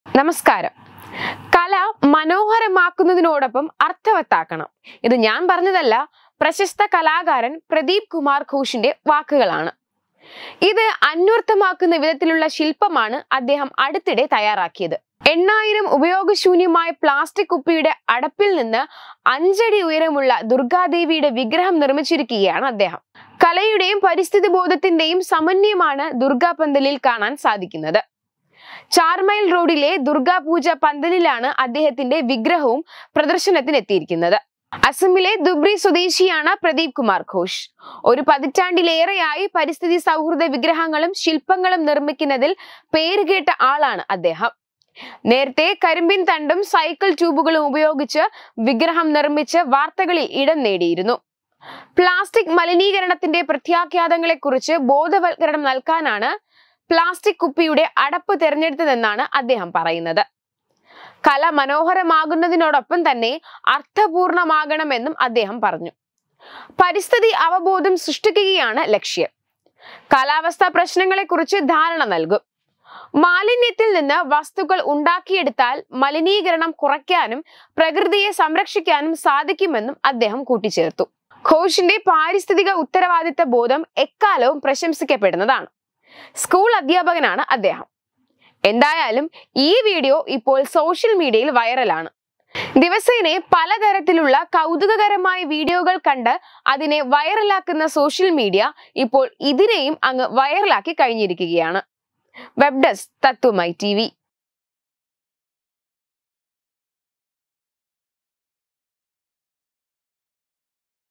"കല മനോഹരമാക്കുന്നതിനോടൊപ്പം അർത്ഥവത്താക്കണം " |DURGADEVI
8,000 പ്ലാസ്റ്റിക് കുപ്പിയുടെ അടപ്പുകൾ! ഉയർന്നത് അഞ്ചടി ഉയരമുള്ള ദുർ‌​ഗാ വി​ഗ്രഹം‌ വൈറലായി വീഡിയോ